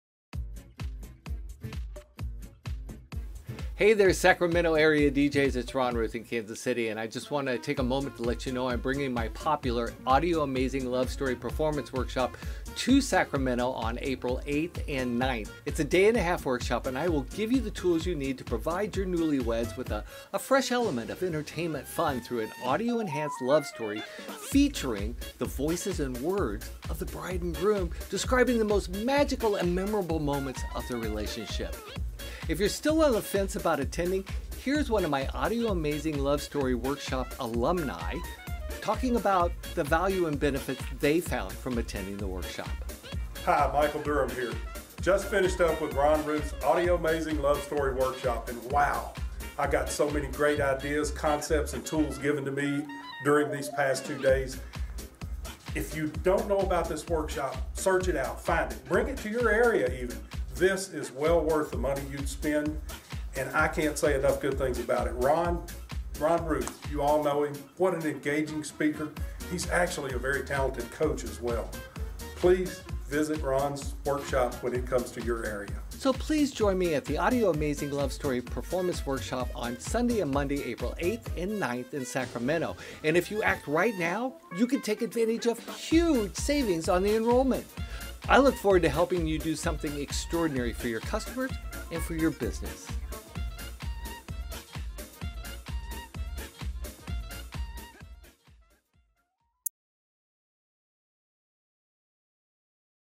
The Audiomazing Love Story Workshop For DJs | Coming To Sacramento
Hey Wedding DJs and Disc Jockeys, The Audiomazing Love Story Workshop is coming to Sacramento, CA, April 8-9, 2018.

Join Ron Ruth, 25-year wedding professional, certified Wedding Entertainment Director® and creator of The Audiomazing Love Story for an intensive learning experience that will be one of the best investments you'll make in yourself and your business! Really.

The workshop curriculum includes the creative secrets and experienced coaching for a fresh element of wedding entertainment fun that your clients and their guests will rave about for years to come---an amazing, audio-enhanced love story presentation featuring the voices and words of your newlyweds describing the most magical and treasured moments of their relationship.

Plus, you'll learn skills that will put you on the fast track to improving your performance, increasing your demand and dramatically boosting your bottom line.

And, if you need added incentive to enroll, all risk has been removed with an "Absolutely Thrilled," money back guarantee. Details on the workshop enrollment page.